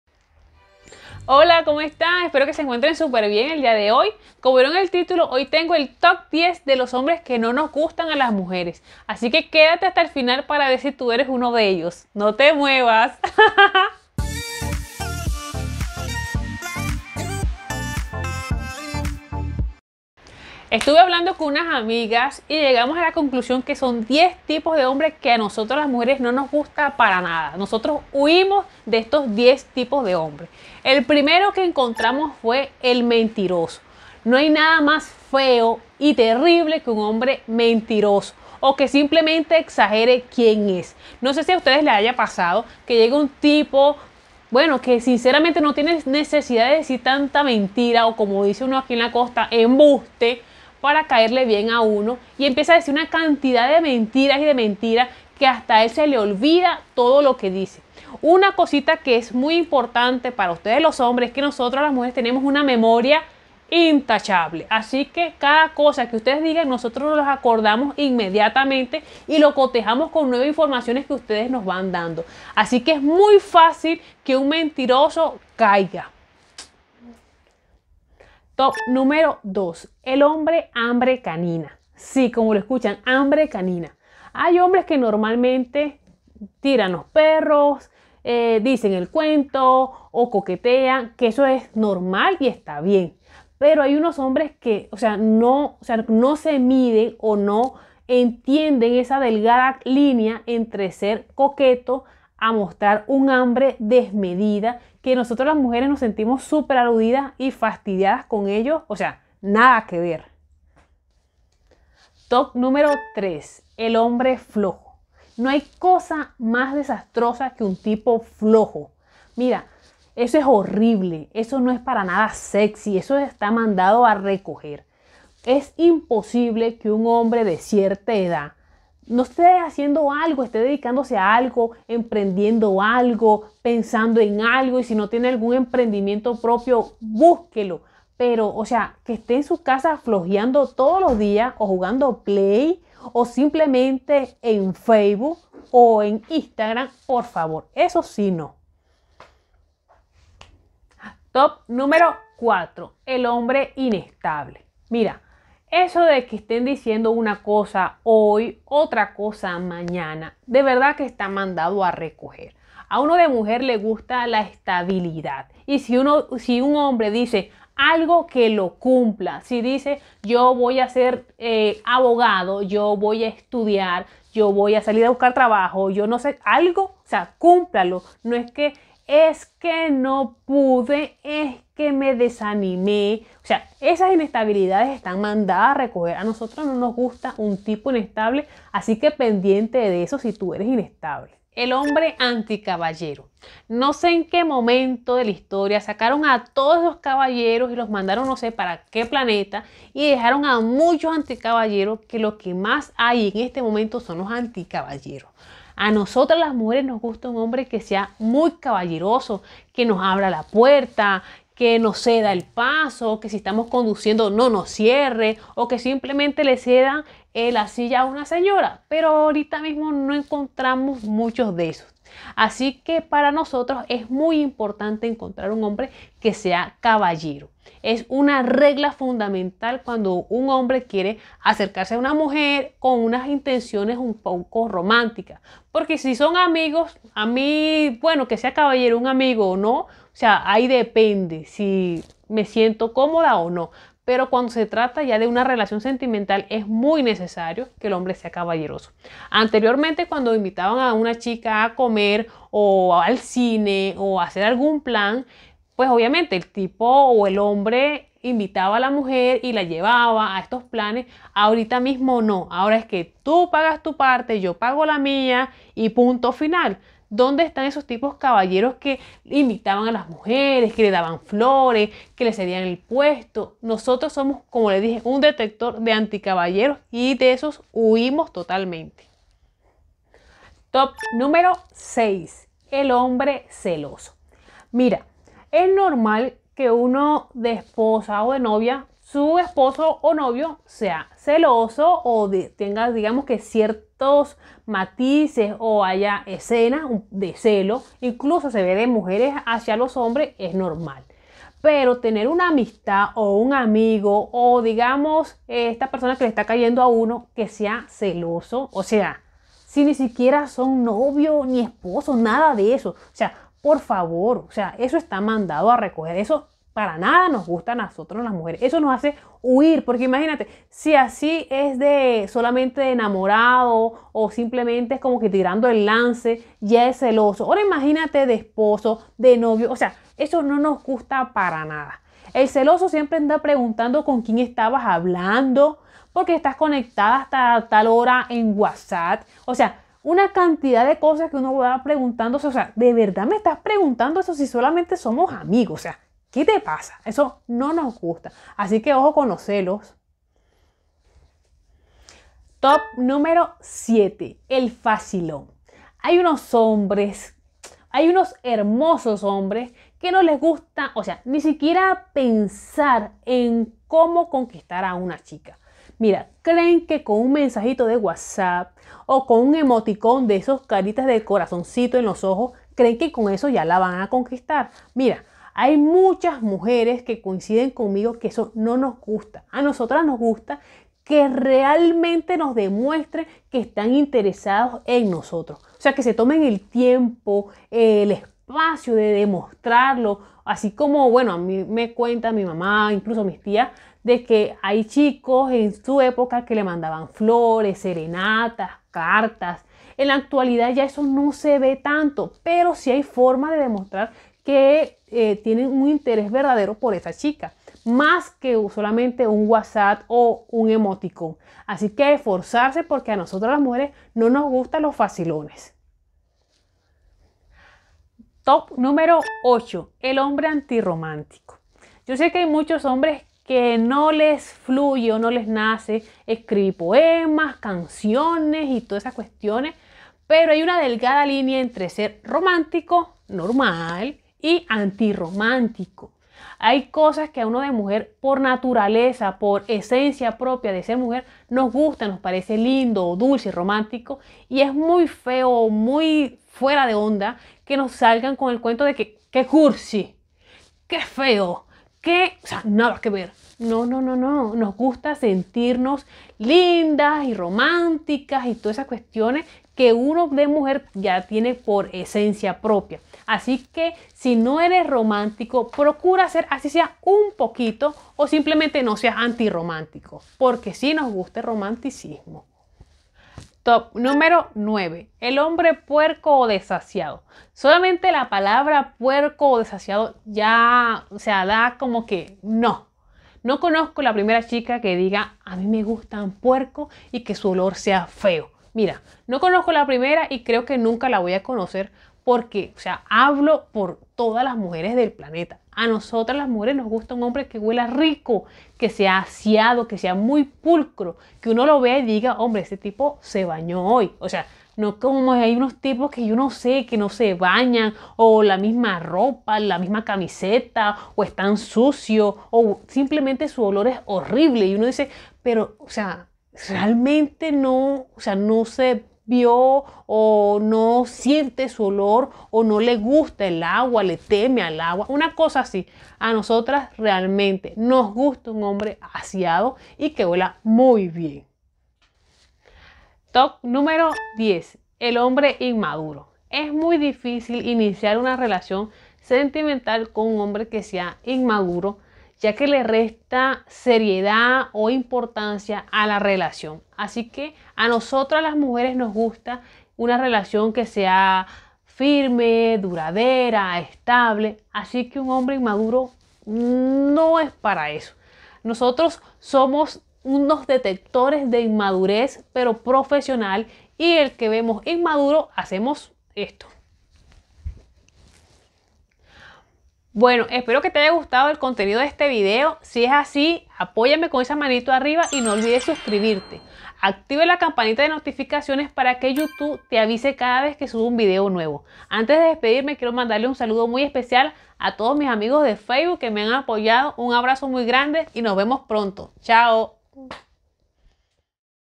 TOP 10 - Hombres que NO nos gustan a las mujeres
Quieres saber como ser más eficaz a la hora de conquistar a una mujer?

Primero quiero que sepas todo aquello que a las mujeres NO nos gusta de los hombres. Partiendo de ello te aseguro que se abrirán muchas oportunidades buenas para ti.

Este TOP 10 es:

#1 El hombre mentiroso
#2 El hombre hambre canina
#3 El hombre flojazo
#4 El hombre inestable
#5 El hombre anti-caballero
#6 El hombre celoso
#7 El hombre facilón
#8 El hombre anti-romántico
#9 El hombre puerco
#10 El hombre inmaduro